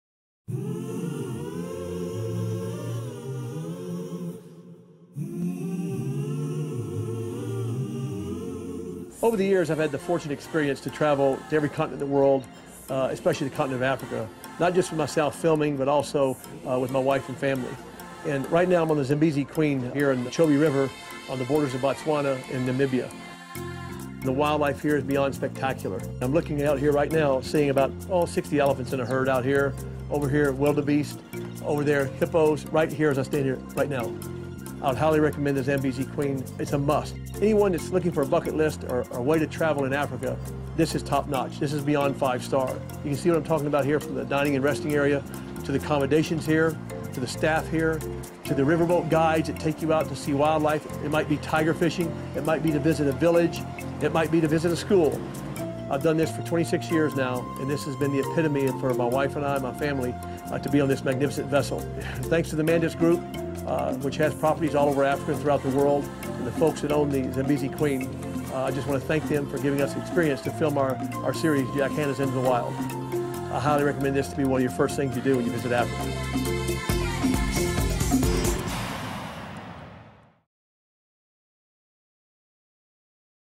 The Zambezi Queen - The New African Safari
AmaWaterways vessel, the Zambezi Queen, takes you on an African adventure of a lifetime. Sailing on the Chobe River in Botswana's Chobe National Park, see the largest population of elephants in the world -- currently estimated at 120,000. That's in addition to prides of leopard and lion, huge groups of buffalo, waterbuck, roan, eland, sable and giraffes. With the ship only carrying 28 passengers, you are guaranteed the best personal experience in Africa.